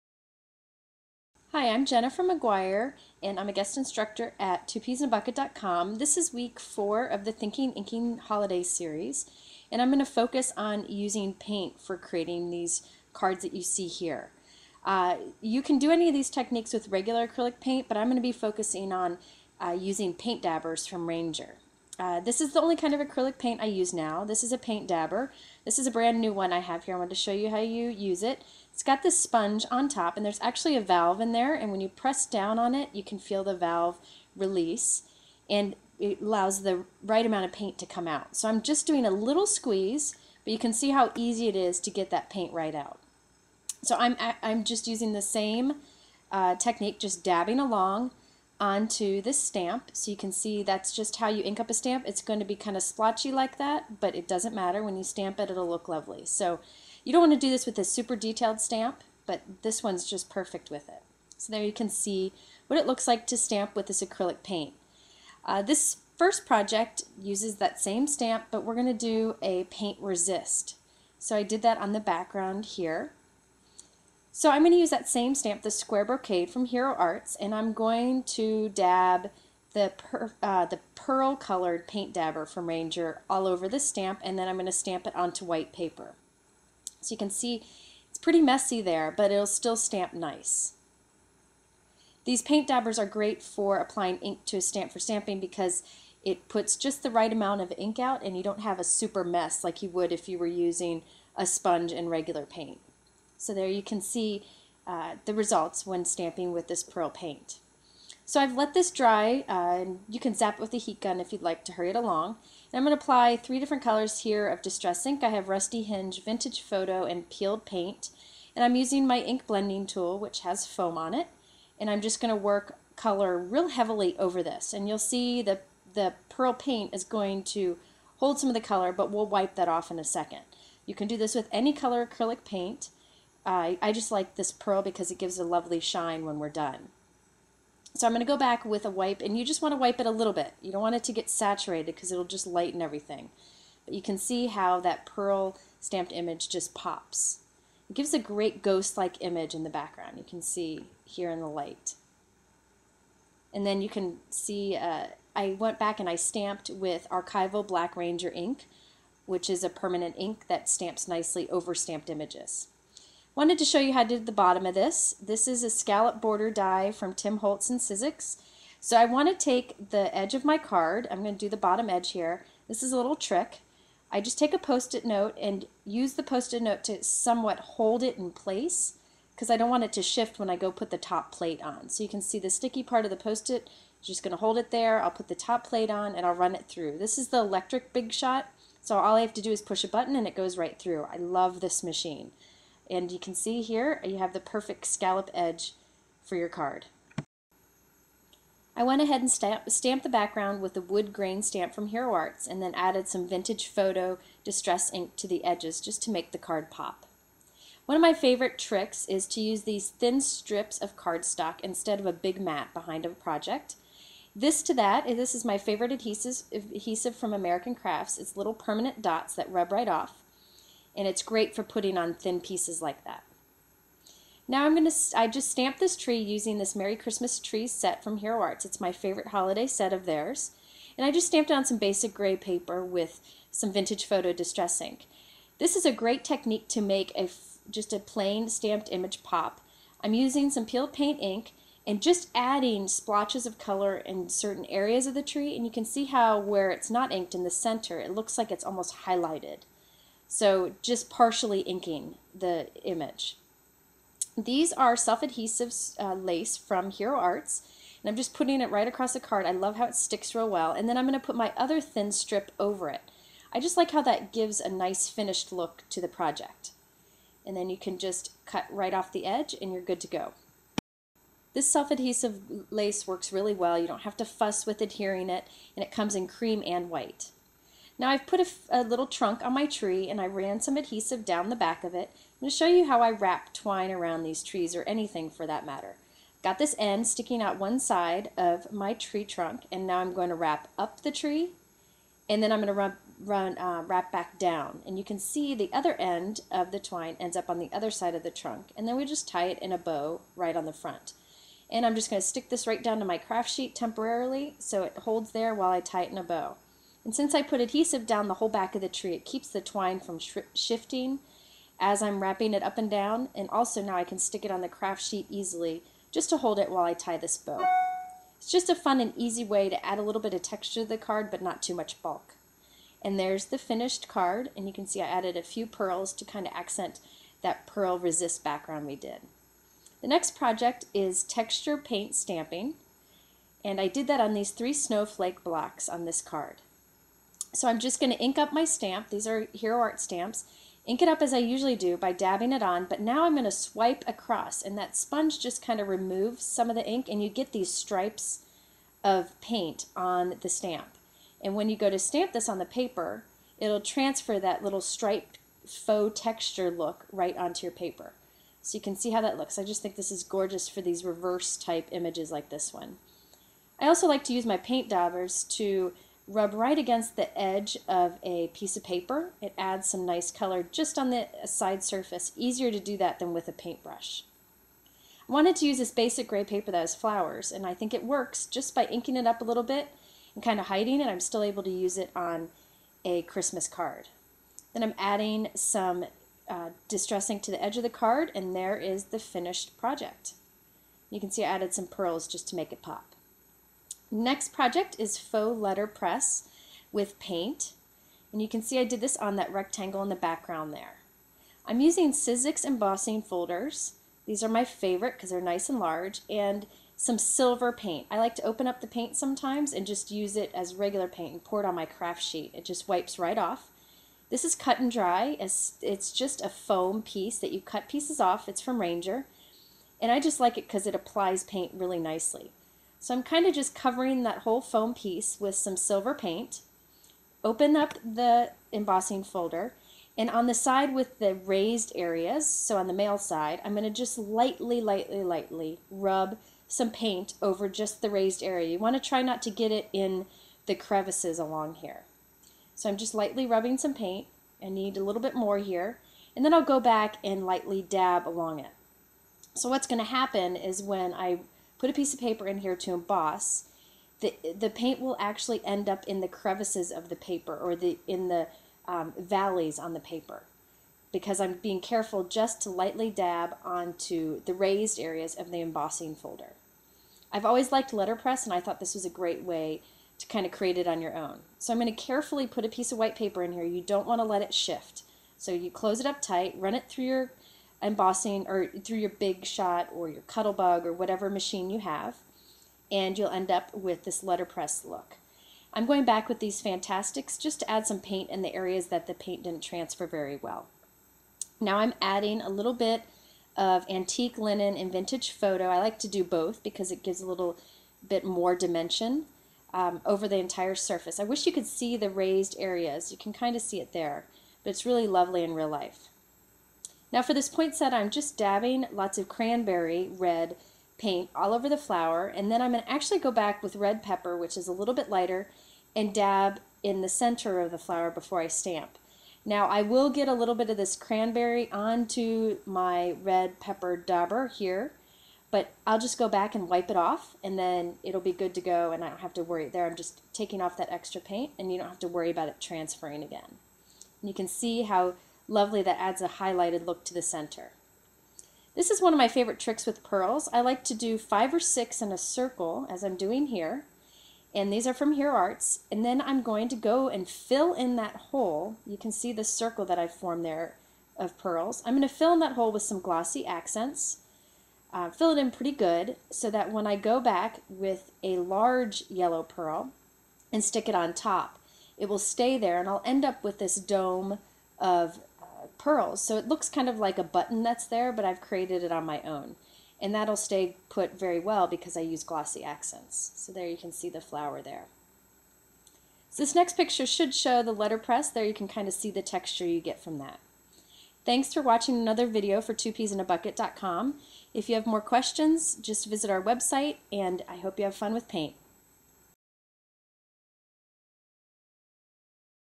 Thinking Inking Week 4 - Paint
This is the 4th in an 8 week online class for twopeasinabucket.com called Holiday Thinking Inking. Watch as Jennifer McGuire show how to use Paint Dabbers from Ranger and Hero Arts stamps to do many different techniques. Also features Sizzix and Basic Grey products. for the October 25th thread about this particular video. There will be additional tips and prizes!